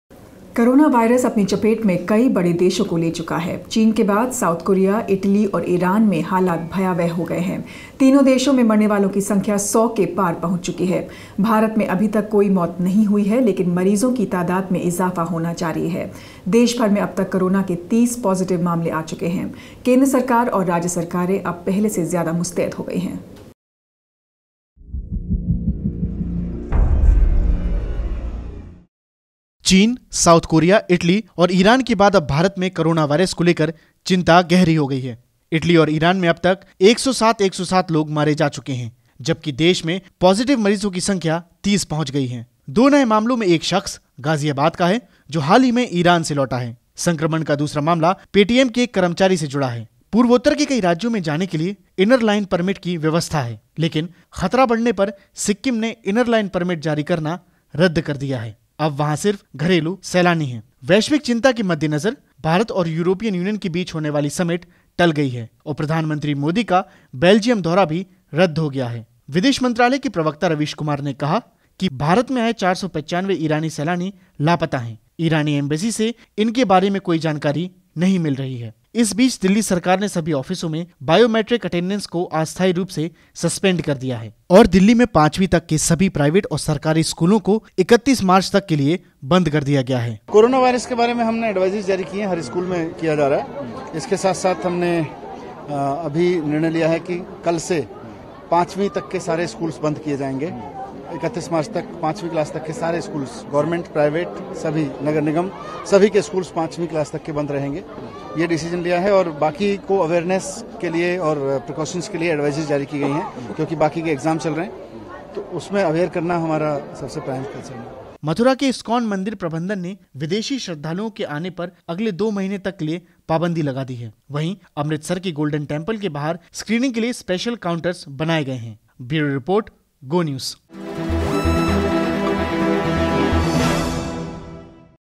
Global Coronavirus Deaths Rise, India Strains To Tackle Crisis
Worldwide death toll from coronavirus rises to over 3,300 with South Korea, Italy and Iran presently the worst affected. India gears up as it stares at a deepening crisis